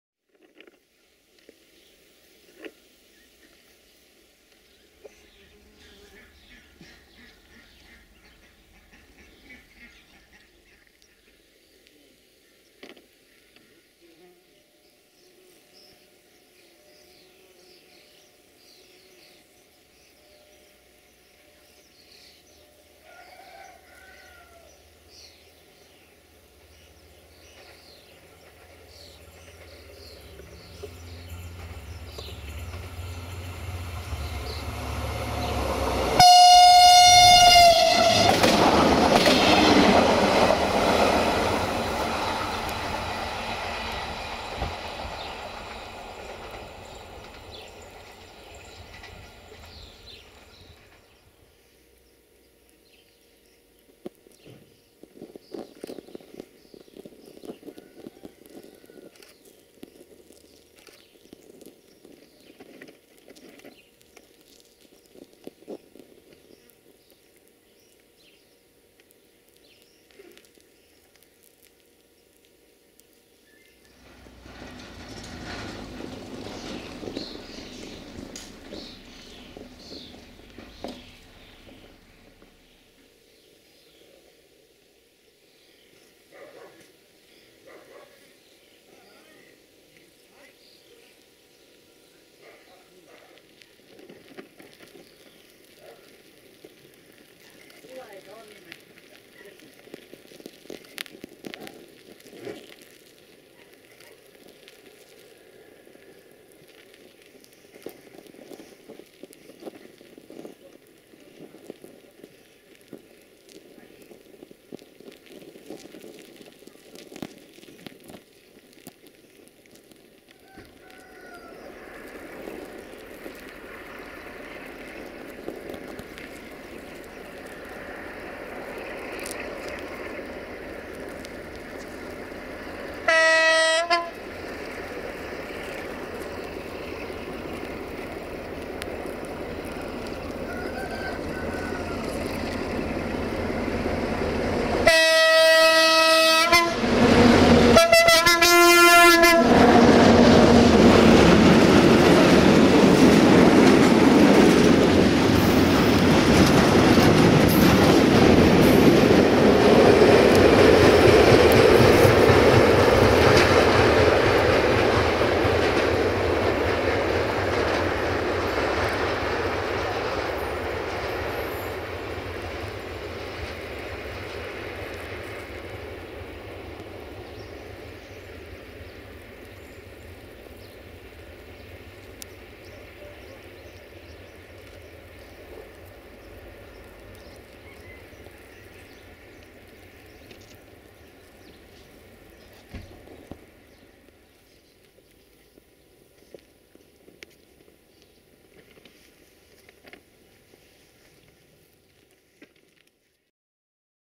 Sulzer Power! DA 60-1526-7 Turat ca la Carte/Full Throttle in Gara Sacadat Station - 12 July 2016
1:IR1533 - Cluj Napoca - Oradea - Desiro VT642 DMU RO-SNTFC
2:Grup Feroviar Roman Freight Train/Marfar/Guterzug - 060-DA/ST43 - LDE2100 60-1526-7 RO-GFR

12 July 2016